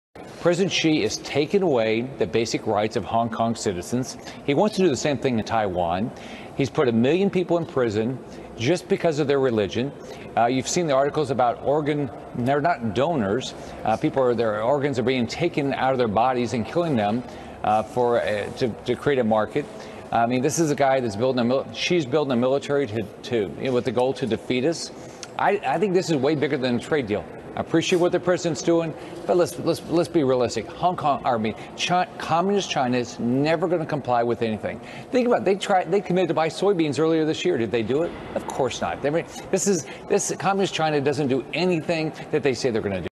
战友之声 參議員 Rick Scott: 從香港問題的處理,到經貿協商,中共不段在欺騙世界所有人
如果您有新兴趣或能力为视频加上字幕,将不胜感激！！！各国语言都可以，谢谢！